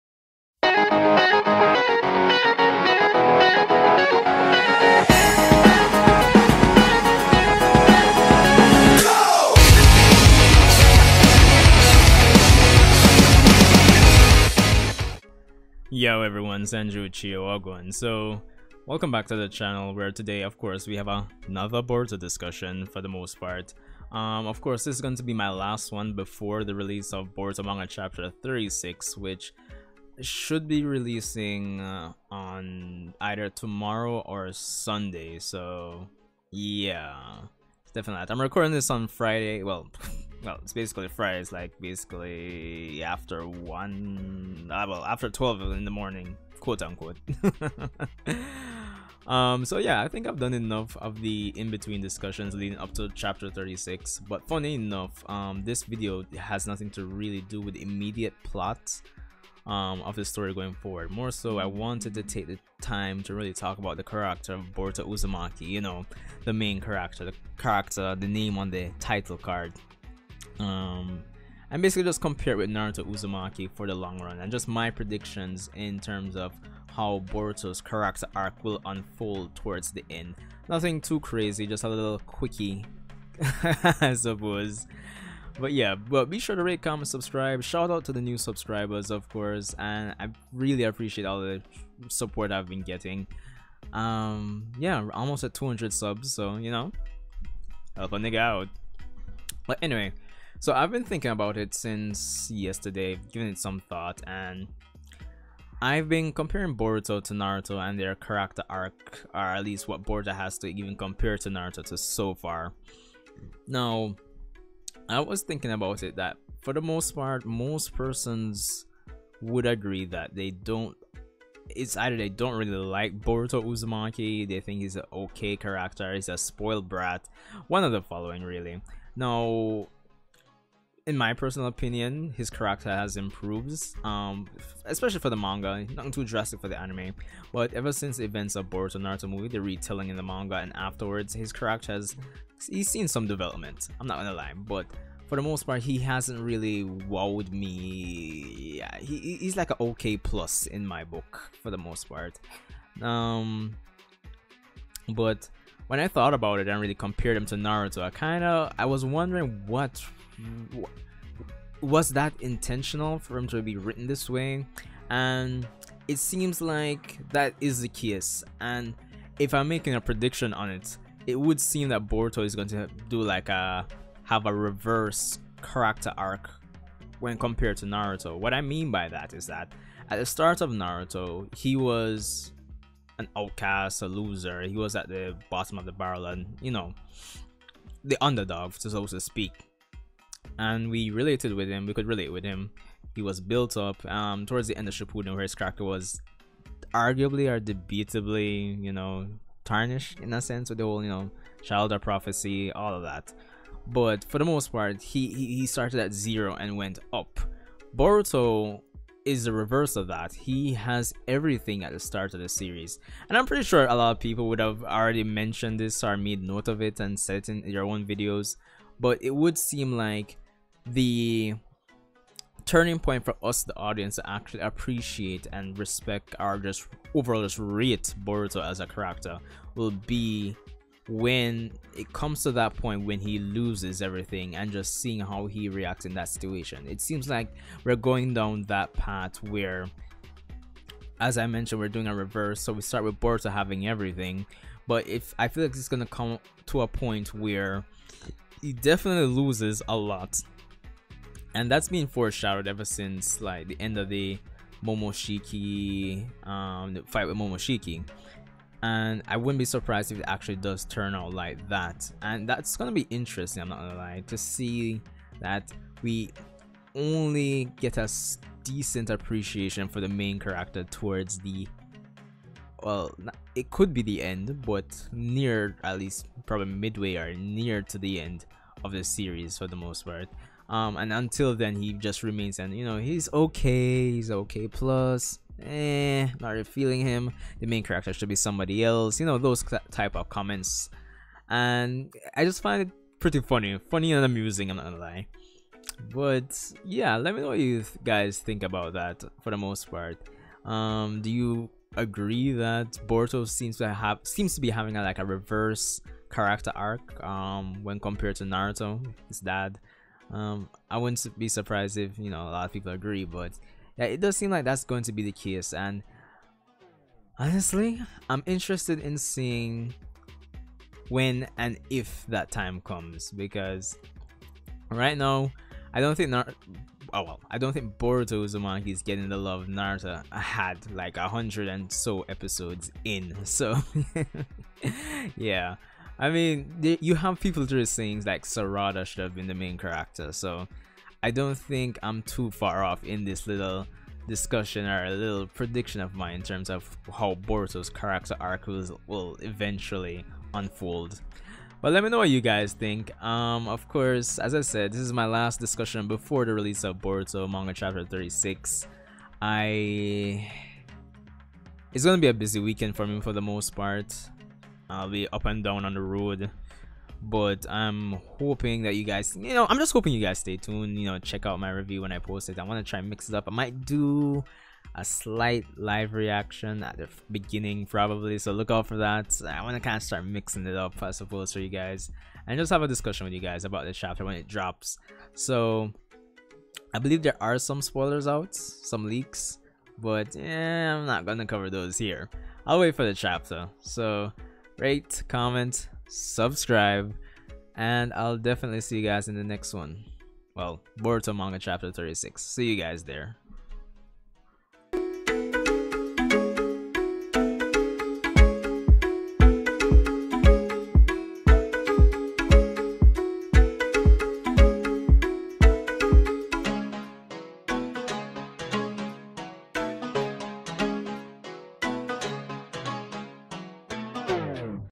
Boruto Uzumaki May Become A Fan Favourite Character at The End
Boruto Uzumaki as a character has gotten a variety of criticisms. Ranging from pushing him aside and making other characters such as Sarada Uchiha or Mitsuki the main character. While there has been growth in his character, he's not at the same point as Naruto in terms of love and support even though the Boruto: Naruto Next Generations Anime is more than 100 episodes in